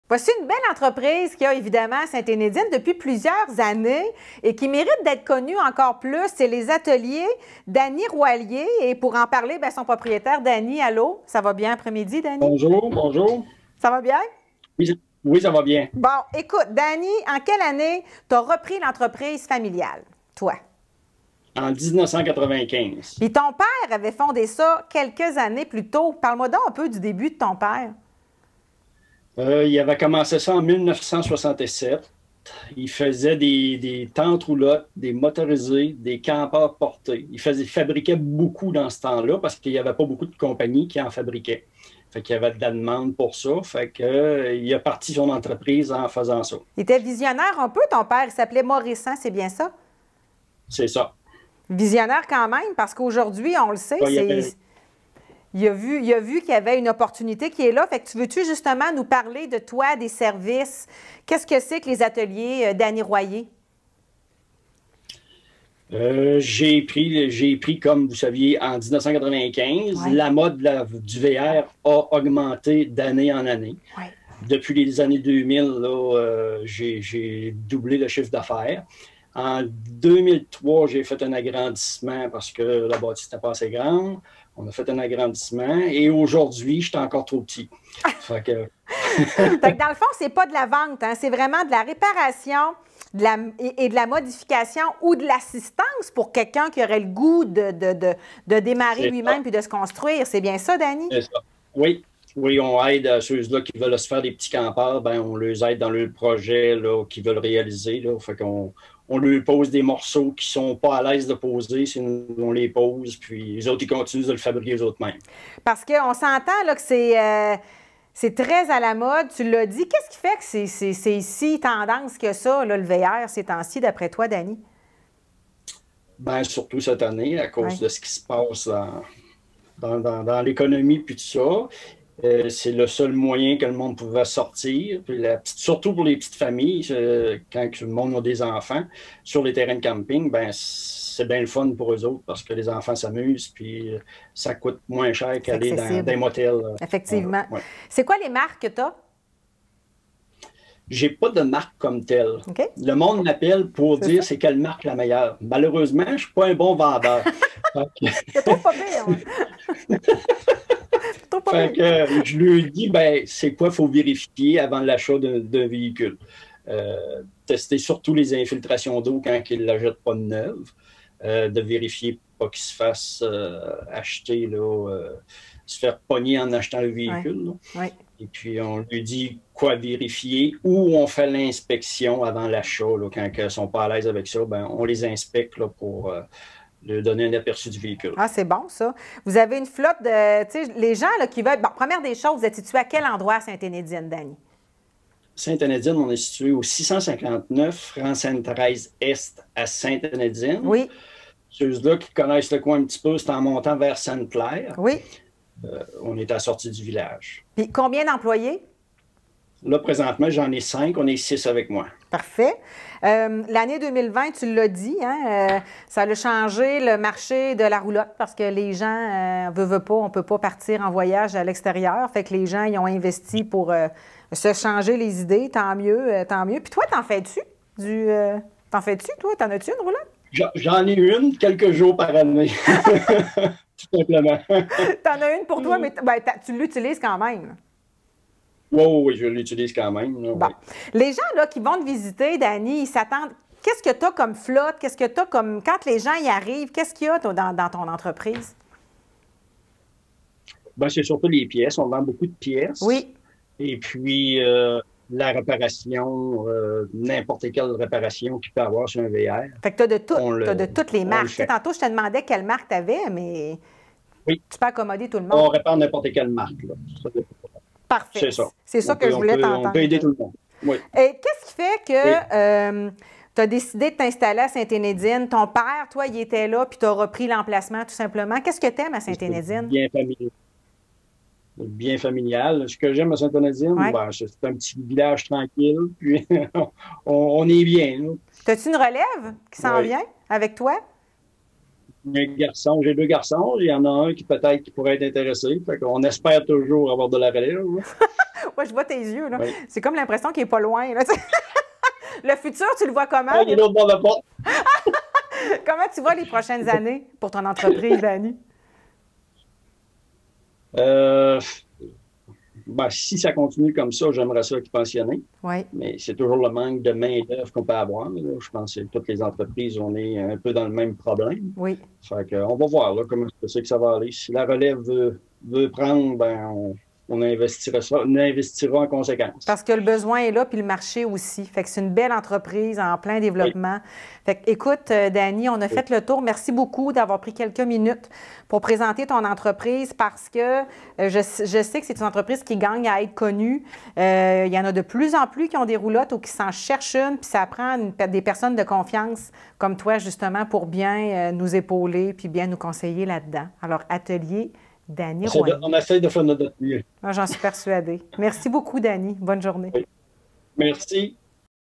Ateliers Dany Royer
Nous vous faisons découvrir la vie économique de chacune des municipalités de la MRC de la Nouvelle-Beauce, dans le cadre de la campagne d'achat local de la Chambre de commerce et d'industrie Nouvelle-Beauce onlaici. Je vous présente les Ateliers Royer de Ste-Hénédine.